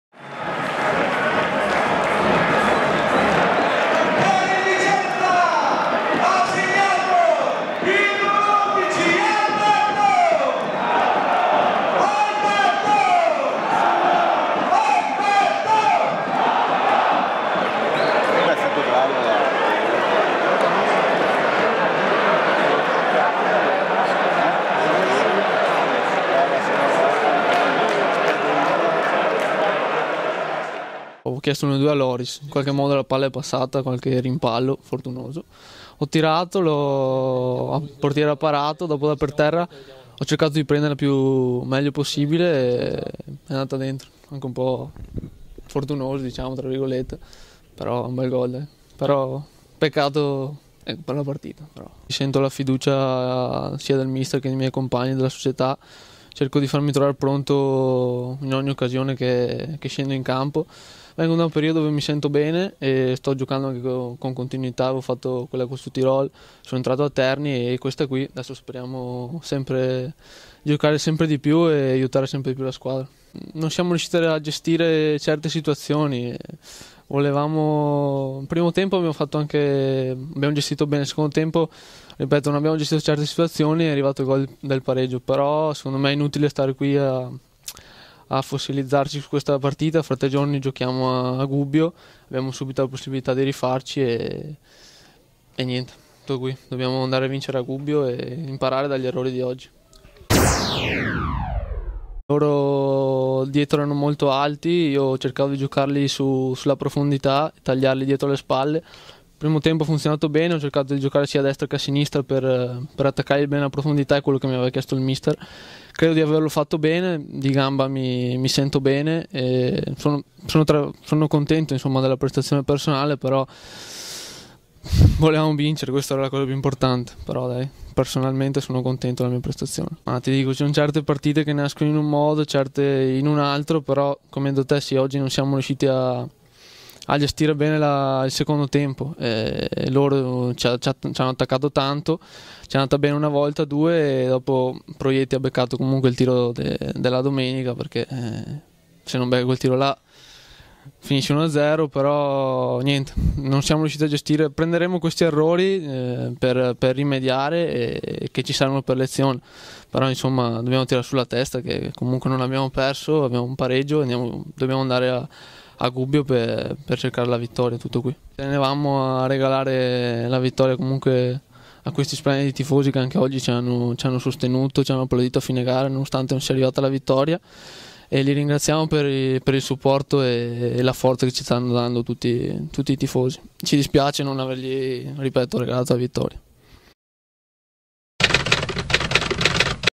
CALCIO - l'autore del gol Tronco dopo il pareggio con Teramo
LR VICENZA - TERAMO, 1-1 (1-0 pt) - L'intervista ad Alberto Tronco oggi goleador per il L.R. Vicenza Virtus ... felice per la rete, per il suo rendimento, meno felice per il pareggio e infime dispiaciuto per non aver dato la gioia piena della vittoria agli 8600 tifosi che hannio seguito il match dagli spalti del glorioso "Romeo Menti" ...